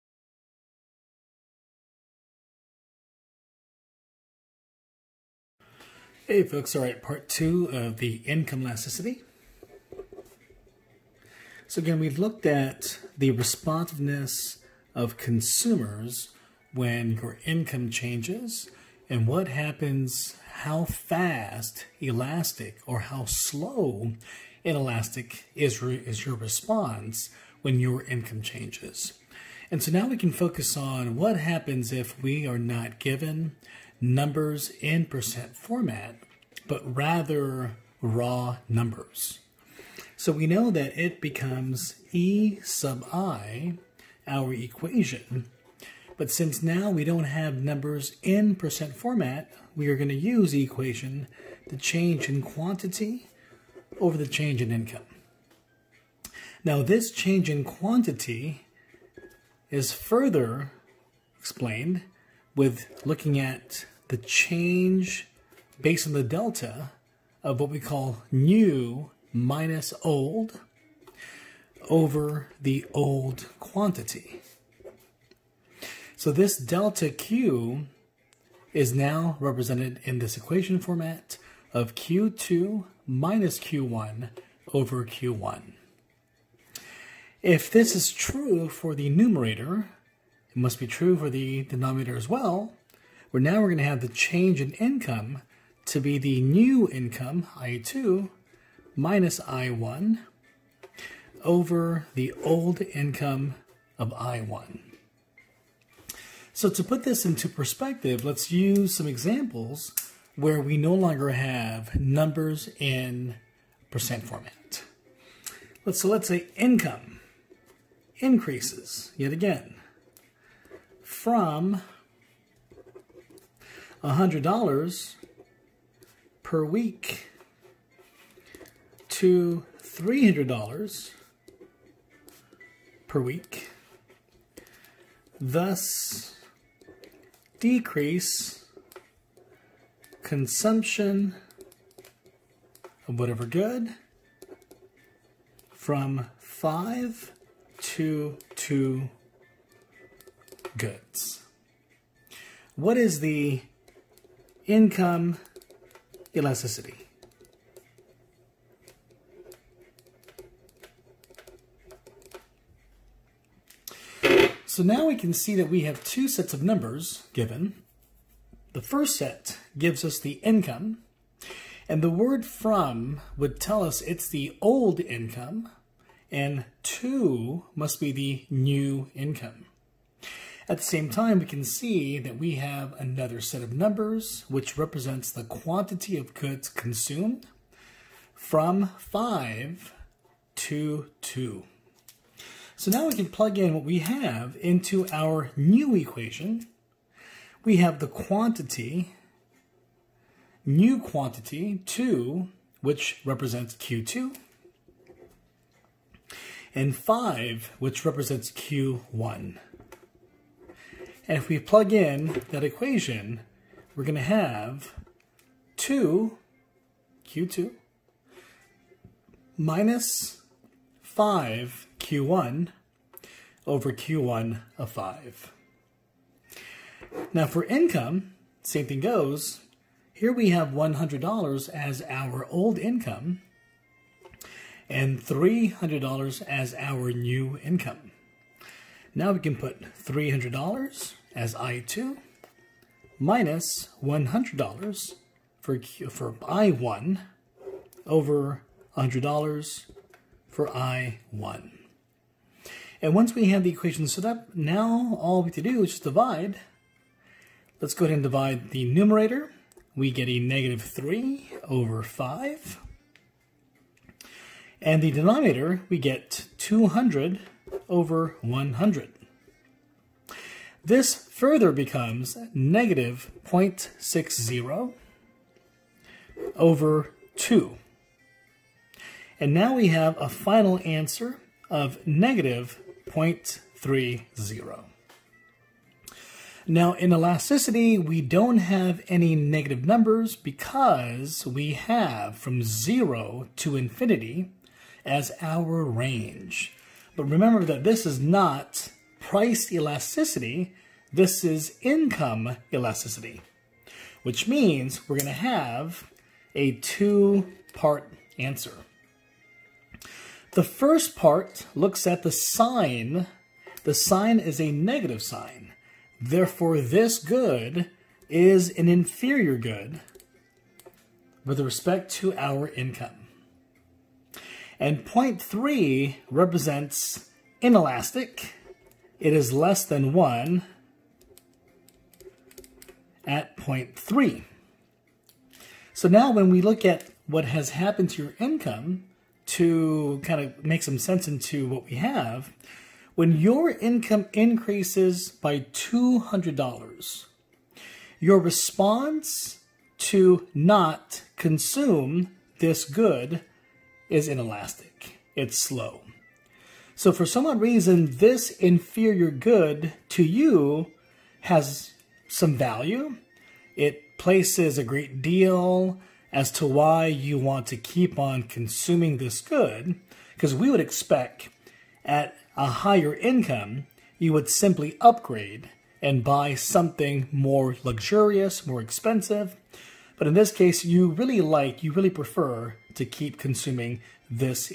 Income Elasticity 2
We will be using the percent change in quantity over the percent change in income equation.